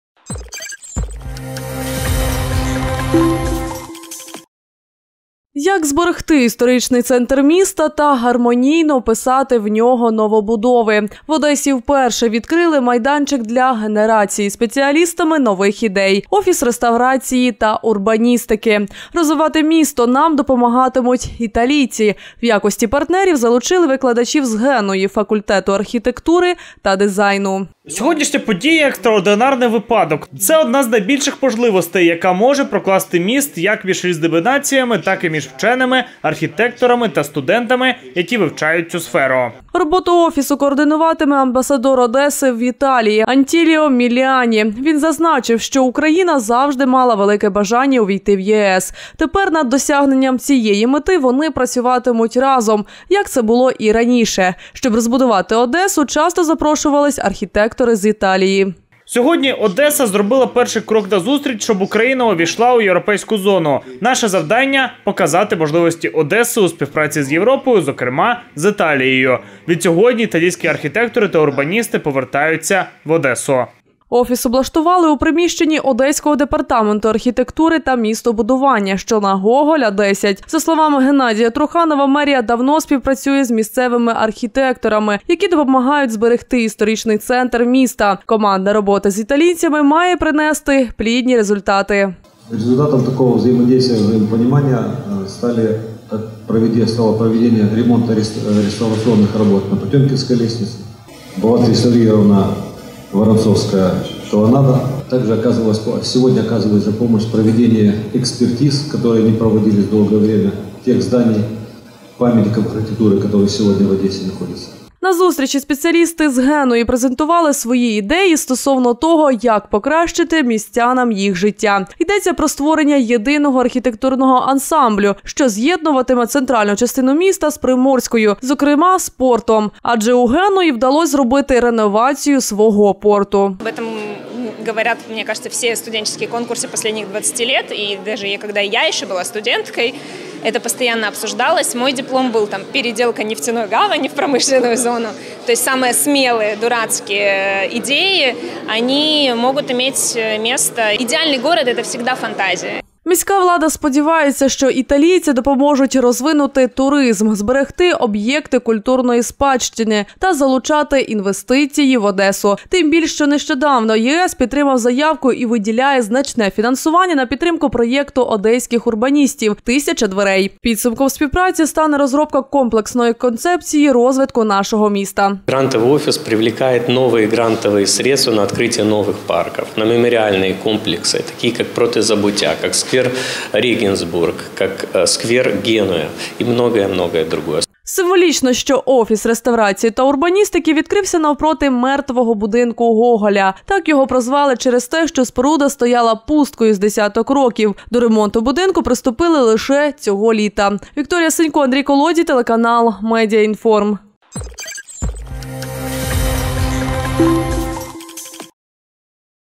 Media_акцент. Одеса - історична та гармонійна
Як зберегти історичний центр міста та гармонійно вписати в нього новобудови?  В Одесі вперше відкрили  майданчик для генерації спеціалістами нових ідей - Офіс реставрації та урбаністики. Розвивати місто нам допомагатимуть італійці